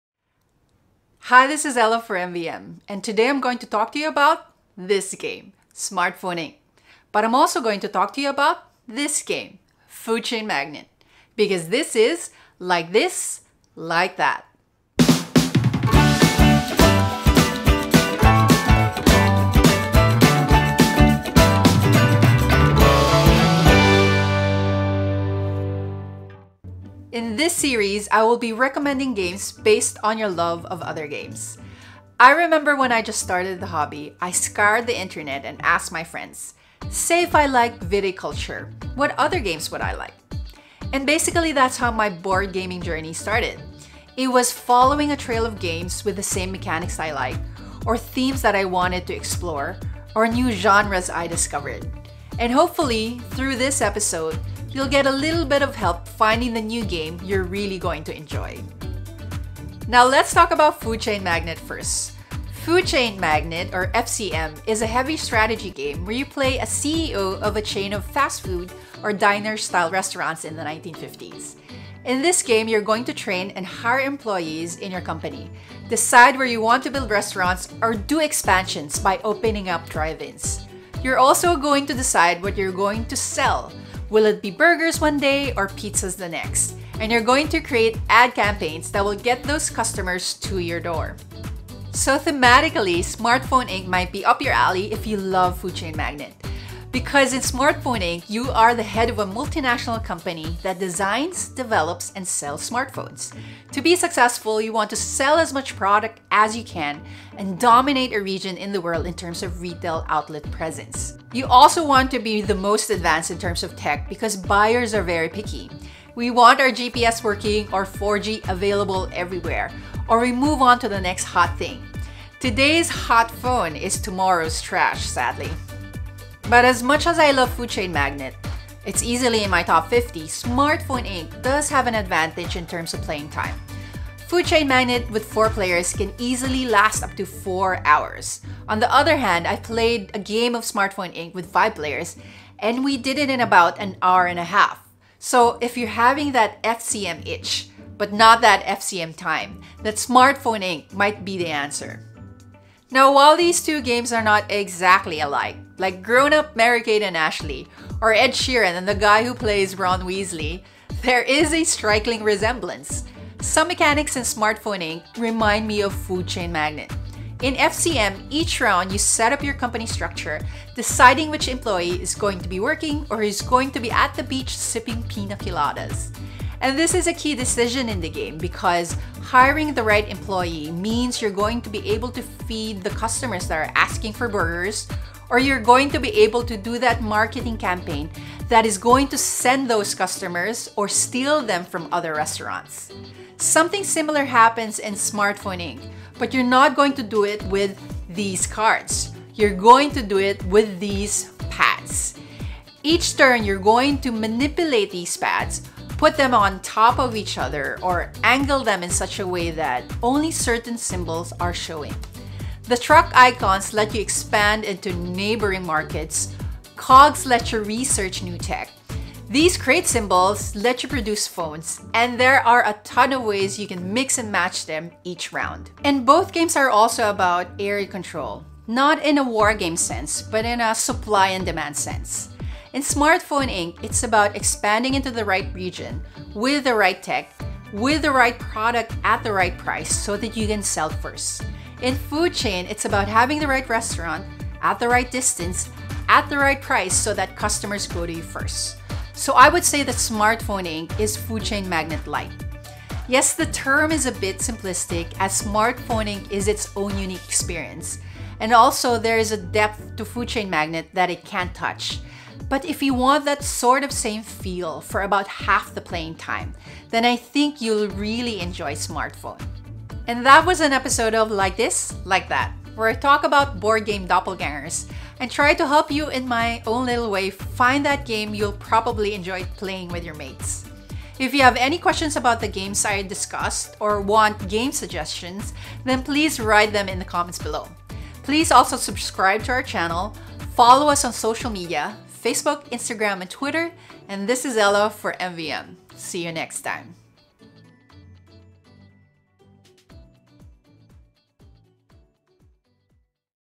Like This/Like That - Episode 1 - by Man Vs Meeple
Like This/Like That is Man Vs Meeple's brand new series from Ella Ampongan!  It's an insightful look at two Top Tier games that may look very different on the surface, but hold common similarities for people who are in search of their next favorite game.

In this episode, Ella looks at why enjoying either Smartphone Inc or Food Chain Magnate may lead you to enjoying the other.

Hosted by Ella Galang Ampongan